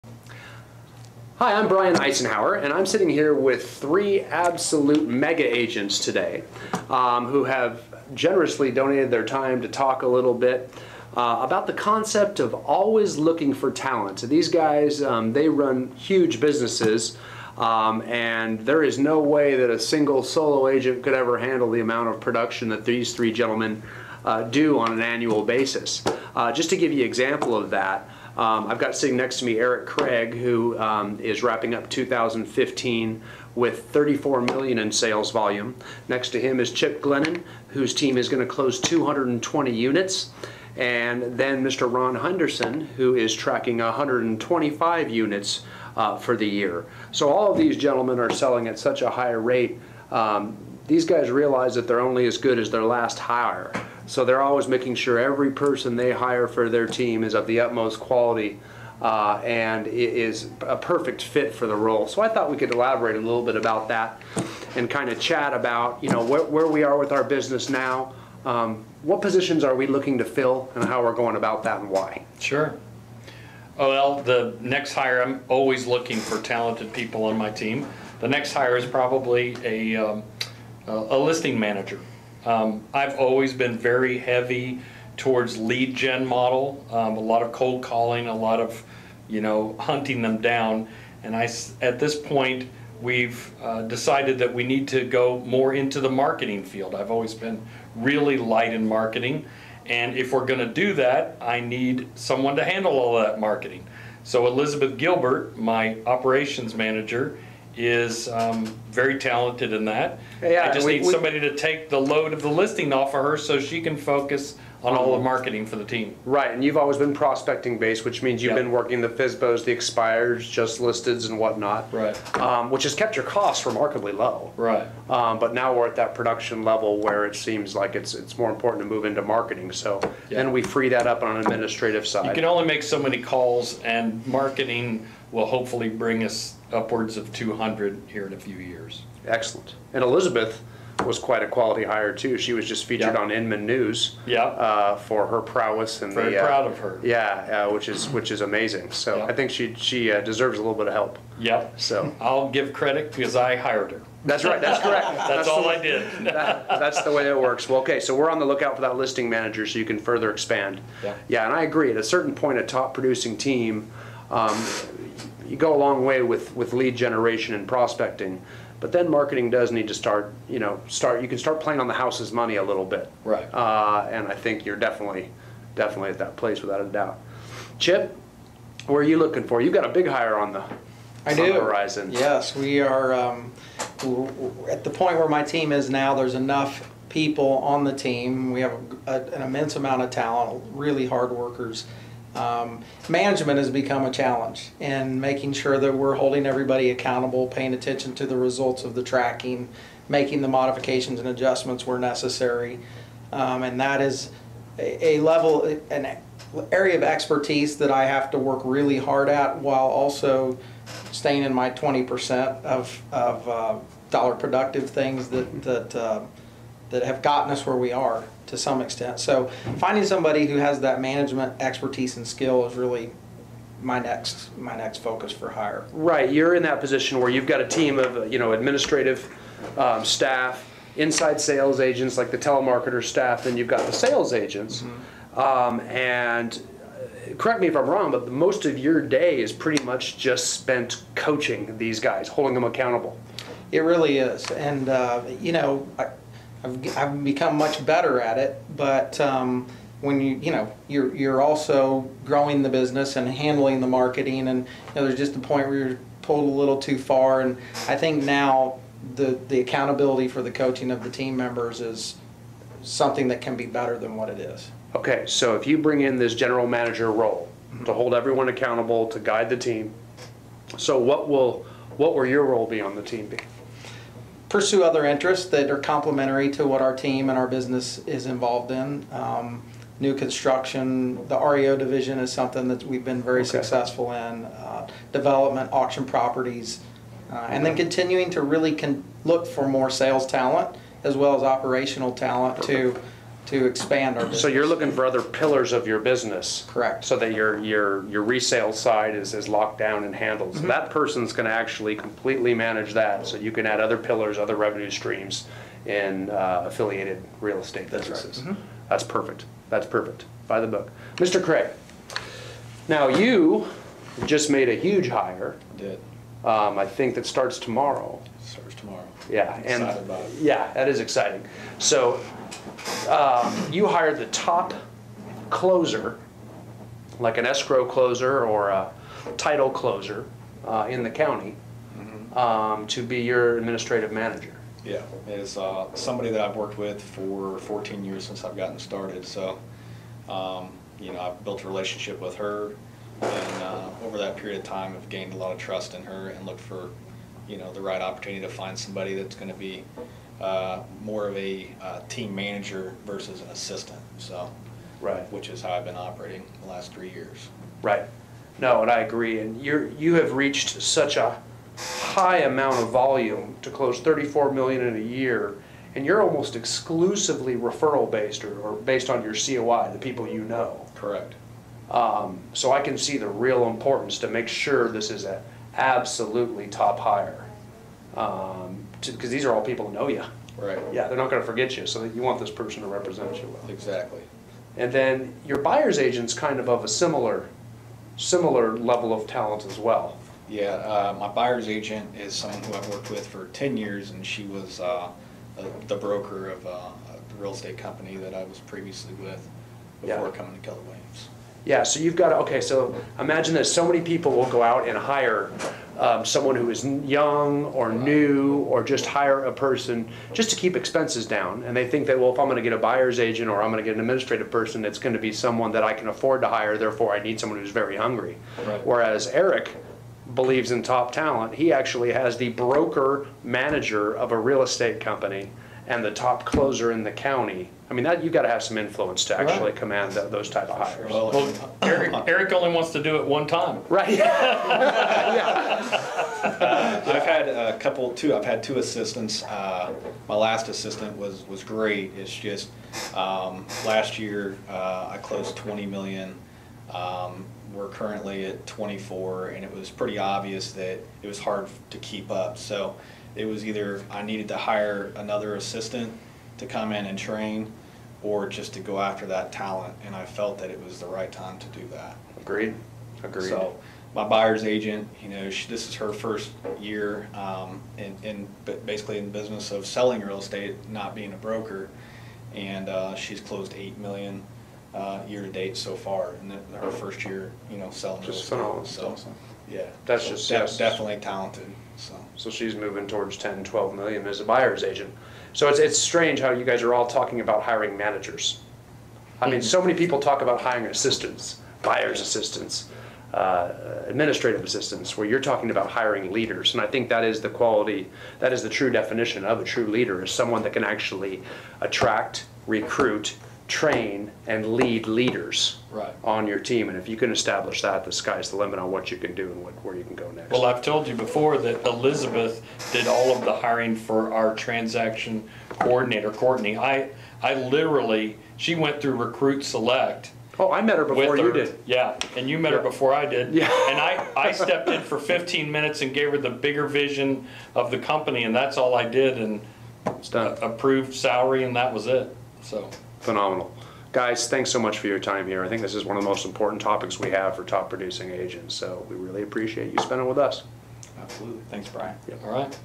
Mega Real Estate Agents - Always Looking for Top Talent
- Mega Agents Ron Henderson, Chip Glennon & Eric Craig discuss how they consistently look for top talent to grow their real estate teams to the next level since they are only as good as their last hires.